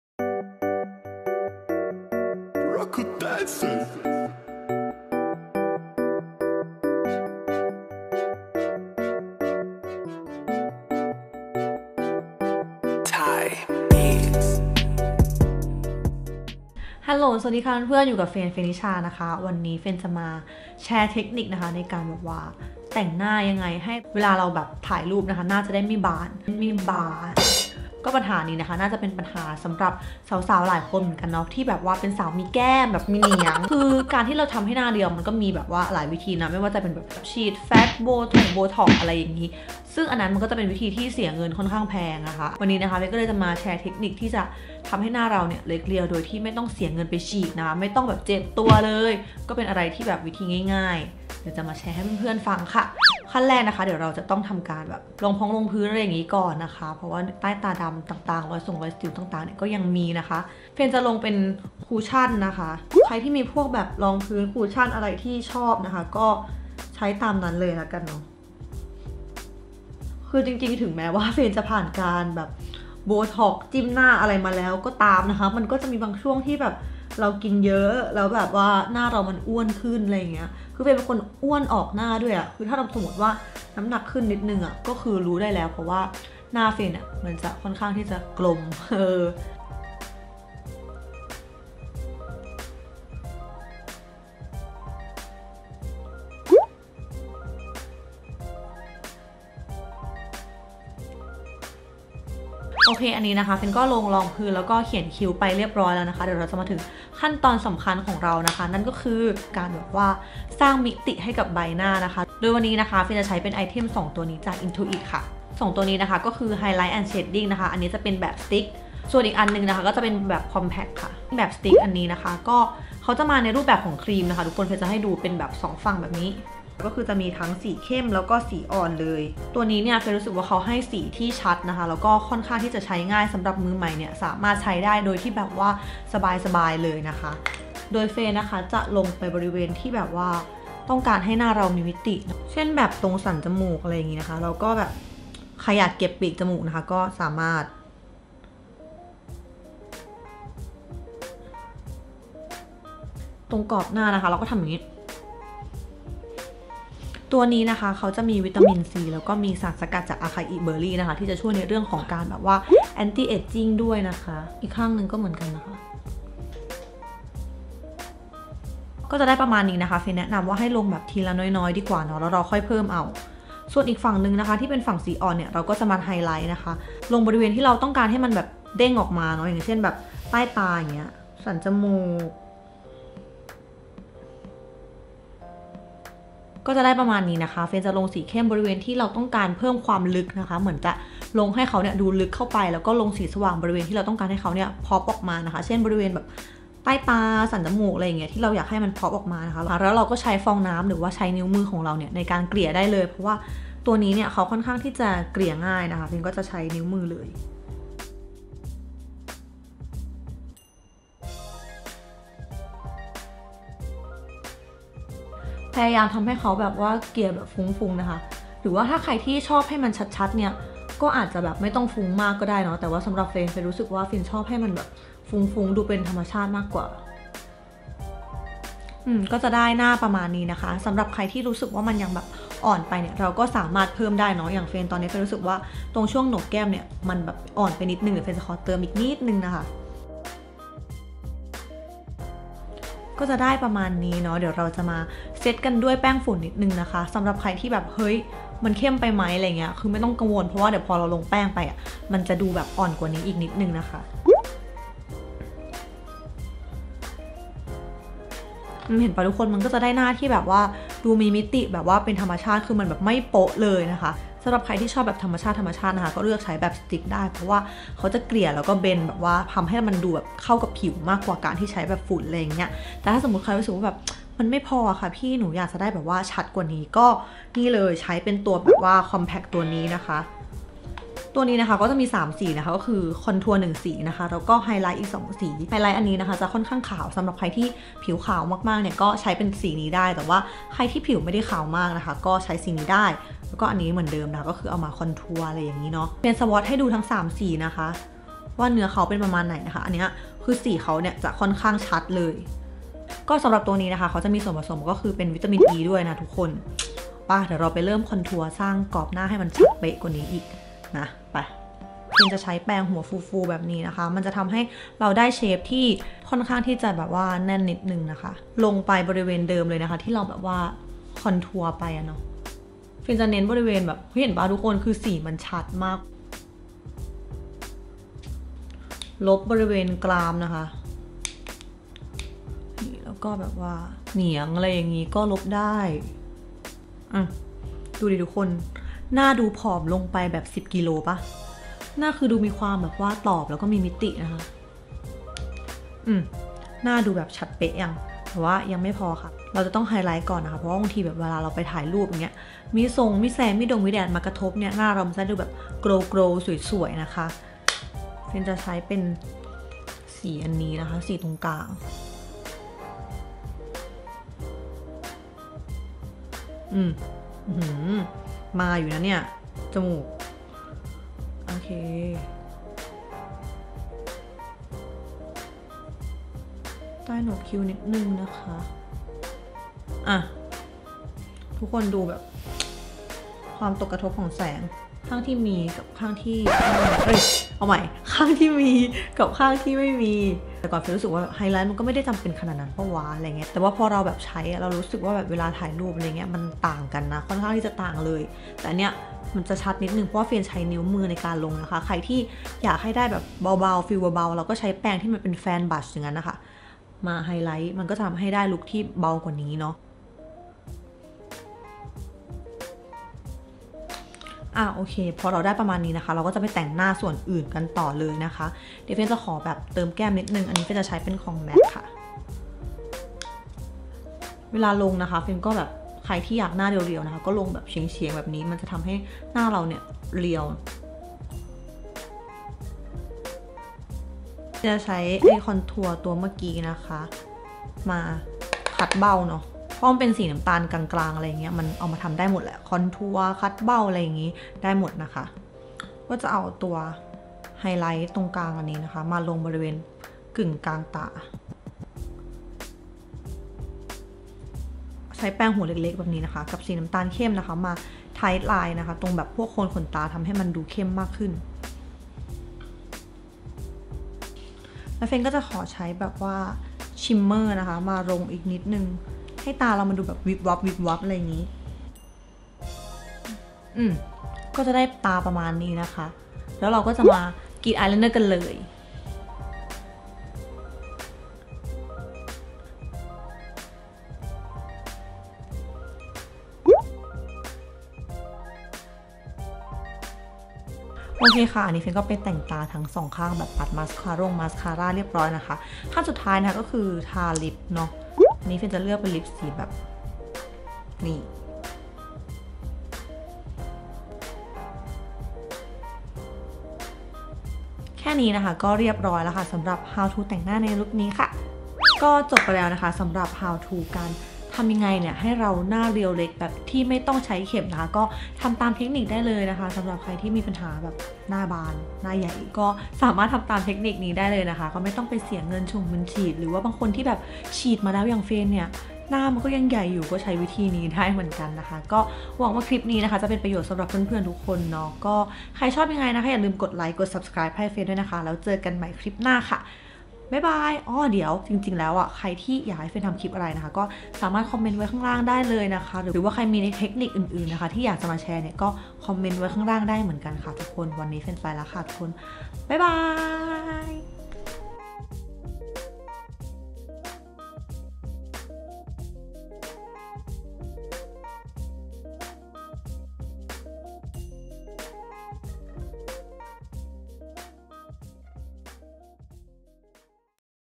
HOW TO หน้าเรียว ดั้งพุ่ง! ถ่ายรูปขึ้นกล้อง l Frennicha
Etude Double Lasting Cushion SPF34/PA++ (890.-)
Cosluxe Slimbrow Pencil Deepbrown (180.-)
IN2IT Highlight&Shading Stick (249.-)
Innisfree No Sebum Powder (270.-)
IN2IT Highlight&Contour Compact (249.-)
MAC Imineralize Blush Happy go rosy (1,400.-)
ESL Magical Eyeliner (259.-)
L’oreal Voluminous Lash Paradise (399.-)
YSL Tint in Balm #06 (1,500.-)